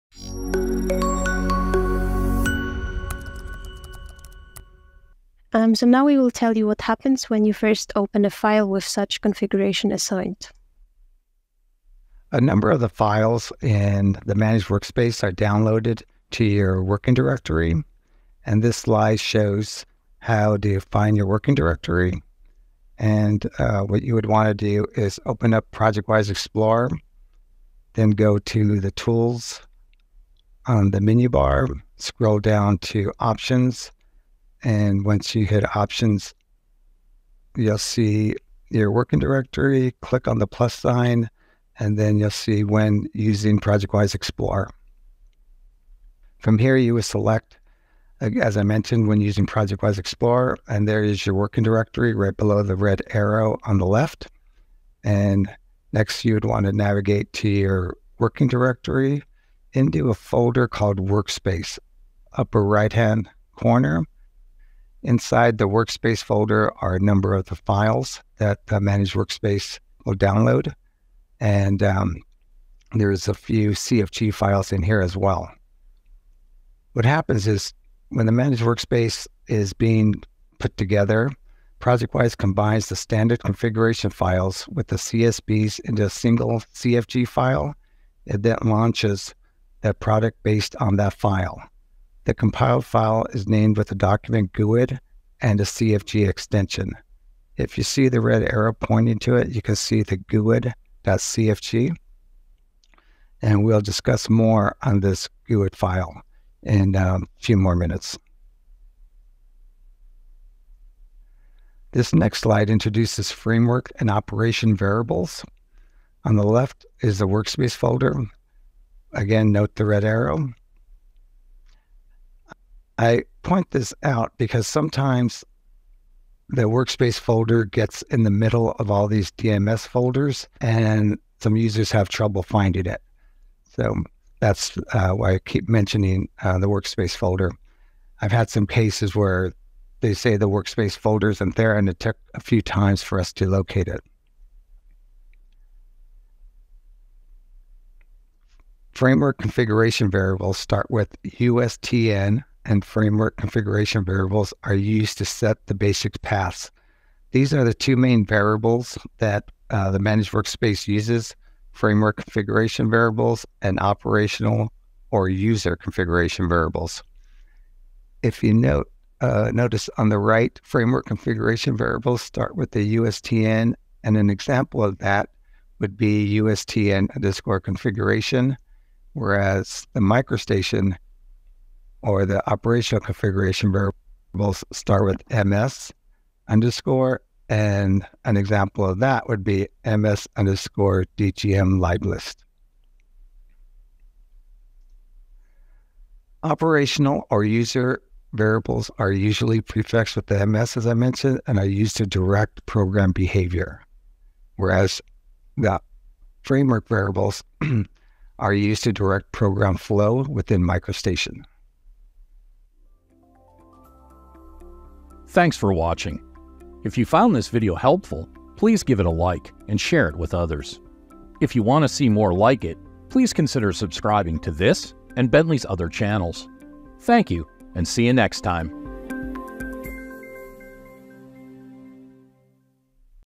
Workspace Folder in the Local Working Directory
A number of the files in the Managed Workspaces are downloaded to your Local Working Directory.

This video shows how to find the Workspace folder in your Local Working Directory and the .cfg file inside it.

Get the latest and greatest product videos, tips & tricks, and product news on the official ProjectWise YouTube channel